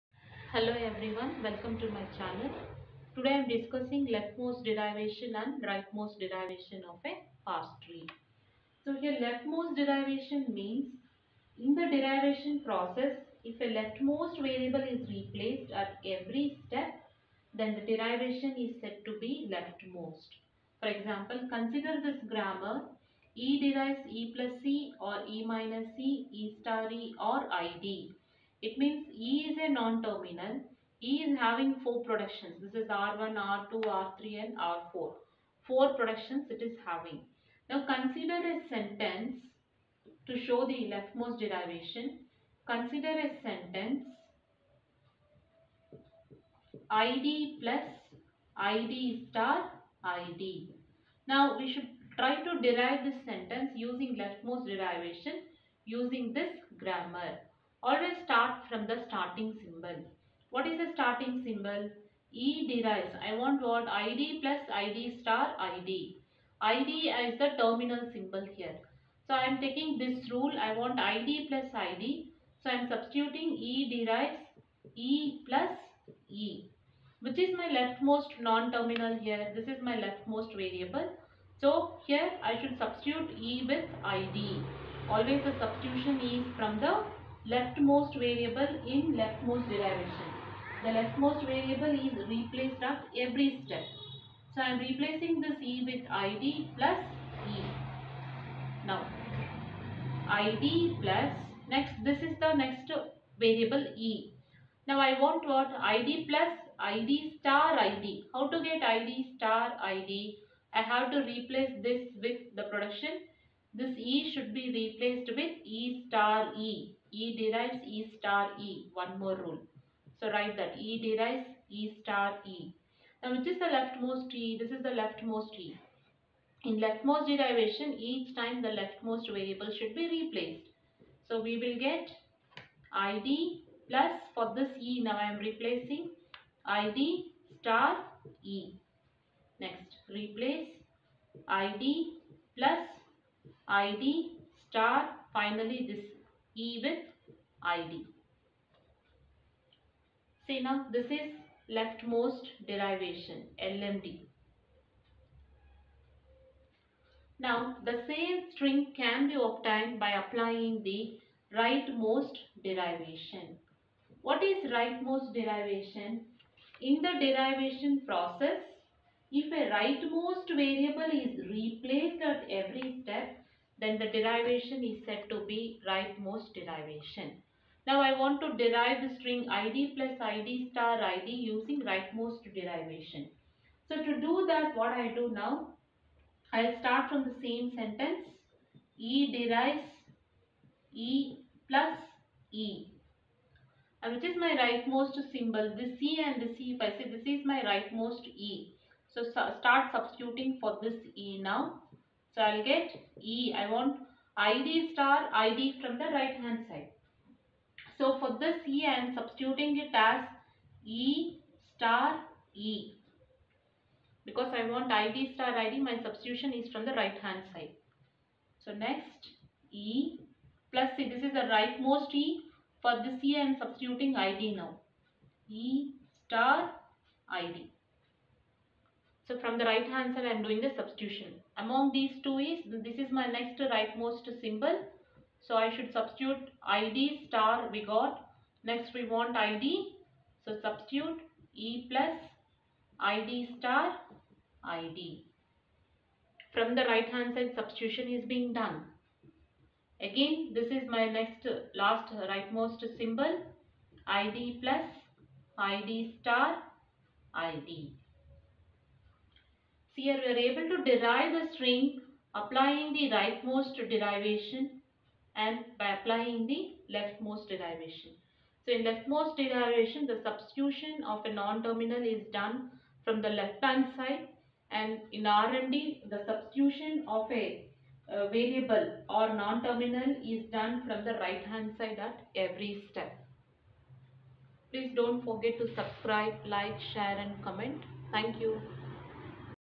Leftmost Derivation and Rightmost Derivationof a Parse tree|LEC-31|ATC VTU syllabus|module-2
Ambiguous grammar ,LMD and RMD of a Parse tree.